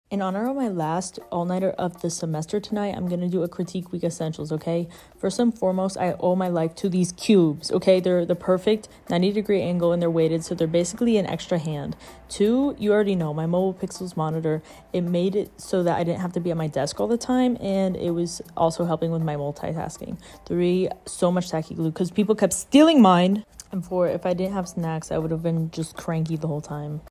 My Critique Week Essentials
Finals week / Day in the life of an architecture student studying at UC Berkely